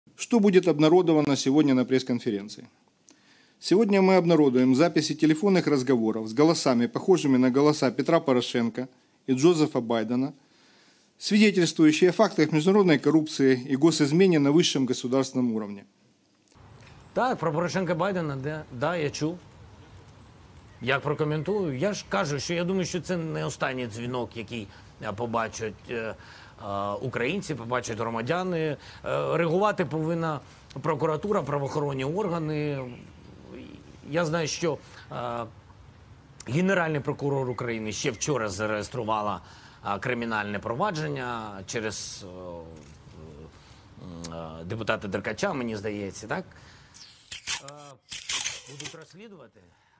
Зеленський - співучасник спецоперації Деркача
Міністерство фінансів Сполучених Штатів розширило перелік осіб та компаній, звинувачених у втручанні у вибори в США. Під санкції потрапив Дубінський та ряд осіб, що публічно з'явилися або пов'язали себе з Деркачем та скоординовано розповсюджували необґрунтовану інформацію про корупційні схеми Байдена.

Матеріали ґрунтуються на аудіозаписах Деркача і Кулика, оприлюднених в травні 2020 року. На пресконференції 20 травня президент Зеленський надав цій інформації офіційного статусу. Також Ірина Венедіктова відкрила кримінальне провадження стосовно аудіоматеріалів, оприлюднених Деркачем. Саме після цього іноземні та українські ЗМІ розпочали масово тиражувати фальшиві твердження. Таким чином спецоперація Деркача за підтримки Зеленського є нічим іншим як спробою втручання на перебіг президентських виборів в Сполучених Штатах.

Очевидно, список громадян України, які потрапили під американські санкції буде збільшуватися.
🎁З чим і вітаємо президента Зеленського!🎁